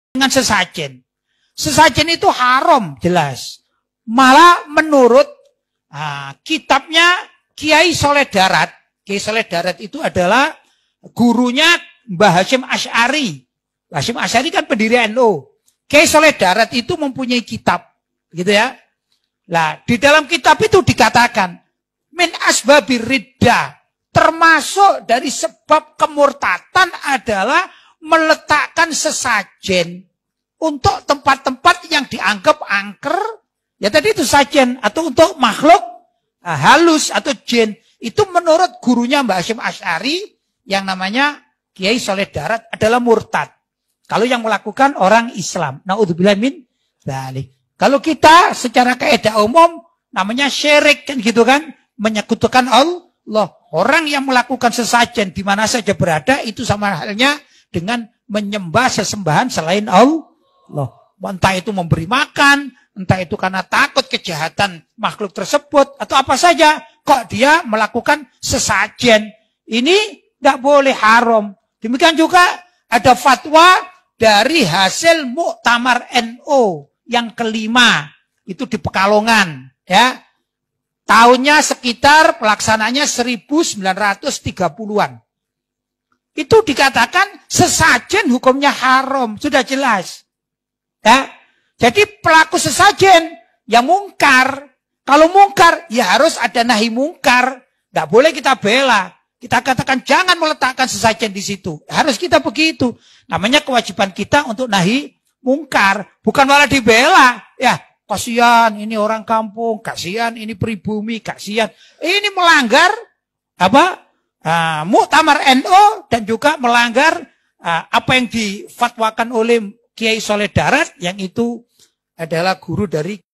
Hukum Sesajen Menurut Nahdhatul Ulama (Ulama NU)
🔰 ADA APA DENGAN SESAJEN. ?

Buku Masalah Keagamaan Hasil Muktamar dan Munas Ulama NU kesatu (1926) sd kedua puluh Sembilan (1994),

Di hasil Muktamar ke-5 tahun 1930 di Pekalongan, masalah nomor 100 dengan judul:

“Perayaan untuk memperingati jin penjaga desa/sedekah bumi, halaman 68-69."

Pertanyaannya:

Bagaimana hukumnya mengadakan pesta dan perayaan guna memperingati jin penjaga desa (mbau rekso Jw.)untuk mengharapkan kebahagiaan dan keselamatan dan kadang terdapat hal-hal yang munkar.

Perayaan tersebut dinamakan “sedekah bumi” yang biasa dikerjakan penduduk desa (kampong, karena telah menjadi adat kebiasaan sejak dahulu kala.”

Jawab:

Adat kebiasaan sedemikian itu hukumnya haram.

Keterangan:
Dalam kitab Jamal ‘Alal al-Jalalaini pada Tafsir Surat Jin
Dalam Kitab Syarah Ihya’ Juz VI (Ithaf al-Sadah al-Muttaqin)

KH.Luthfi Bashori berkata:
"Menurut kitabnya Kiai Soleh Darat, guru dari Kiai Hasyim Asy'ari pendiri NU, muslim yang pasang sesajen itu MURTAD... "